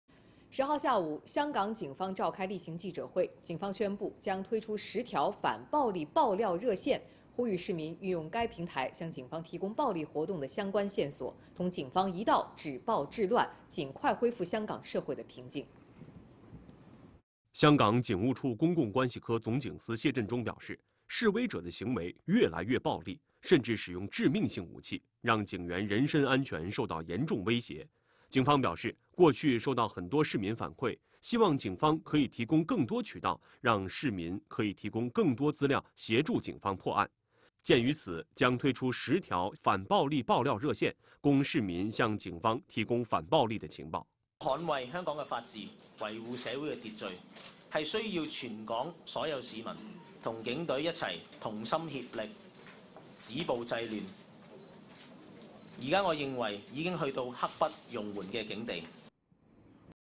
香港警方将推出10条反暴力报料热线 | CCTV中文国际
10日下午，香港警方召开例行记者会。警方在记者会上宣布，将推出10条反暴力报料热线，呼吁市民运用该平台向警方提供暴力活动的相关线索，同警方一道止暴制乱，尽快恢复香港社会的平静。

近两个月来，在香港发生的示威游行活动完全超出集会、游行、示威自由的范畴，已经演化为极端暴力行为，而且手段不断翻新、烈度不断升级、破坏性不断加剧，令人触目惊心。一小部分激进示威者以警察为袭击目标，使用的工具从砖头、铁枝发展到烟雾弹、汽油弹、弓箭等致命武器，并实施纵火，还有人在网上公然鼓吹使用致命手法袭击警察。这些行为对香港警务人员和广大市民的生命财产安全造成严重伤害。已有461人在暴力冲突中受伤，其中包括139名警务人员。更令人愤慨和需要高度警觉的是，部分激进分子撕毁香港基本法，污损国徽。8月3日傍晚，几名无耻之徒在尖沙咀拆下某建筑物前悬挂的中华人民共和国国旗，扔入海中。这一行径严重触犯法律，公然冒犯国家尊严，挑战“一国两制”原则底线。8月5日，反对派发动罢工，造成约250次航班被取消，观塘等8条地铁线停止服务超过5小时，红磡隧道和数条交通干线受阻。极端暴力分子向警署投掷燃烧弹，在多处纵火。当晚7点多，极端激进分子再次到尖沙咀扯下国旗，扔入海中。这些暴徒可谓嚣张至极，可谓丧心病狂。未来等待他们的将是法律之剑的重击，是耻辱柱上抹不掉的刻痕。

持续近两个月的游行示威活动特别是暴力冲击行径对香港经济民生也已造成严重影响。根据特区政府数据，香港今年头六个月各项经济指标，除个别行业外，全面下跌。香港第二季度本地生产总值较去年同期仅实质增长0.6%，经济面临明显的下行压力。已有18个国家和地区因当前情况而对香港发出旅游安全提醒。零售业、旅游业首当其冲，商户销售额和酒店入住率大幅下跌。更严重的是，香港当前事态已严重打击国际投资者的信心。他们已公开表达了对香港营商环境的深切忧虑。有国际评级机构发出预警，持续的游行示威活动和暴力冲击事件使香港经济活动无法正常进行，香港的评级可能受到影响。

必须明确指出，在香港当前乱局中，站在前台的是部分激进暴力分子，居中的是部分被误导、被裹挟的善良市民，但幕后怂恿、支持激进暴力分子的则是香港内外的“反中乱港”势力。他们明目张胆地为激进暴力分子撑腰壮胆，明里暗里进行协调、指导和资助。他们混淆是非、颠倒黑白，竭力鼓吹所谓“公民抗命”甚至“暴力才能解决问题”的歪理邪说，美化、怂恿激进暴力活动。他们鼓动激进分子大搞所谓“不合作运动”，实施“极限施压”，企图迫使全体香港市民卷入政治纷争，激化社会矛盾。

铁一般的事实显示，这场持续了近两个月的激进示威活动性质十分严重、影响十分恶劣，已经对香港繁荣稳定造成严重影响，正在把香港拖向危险的深渊！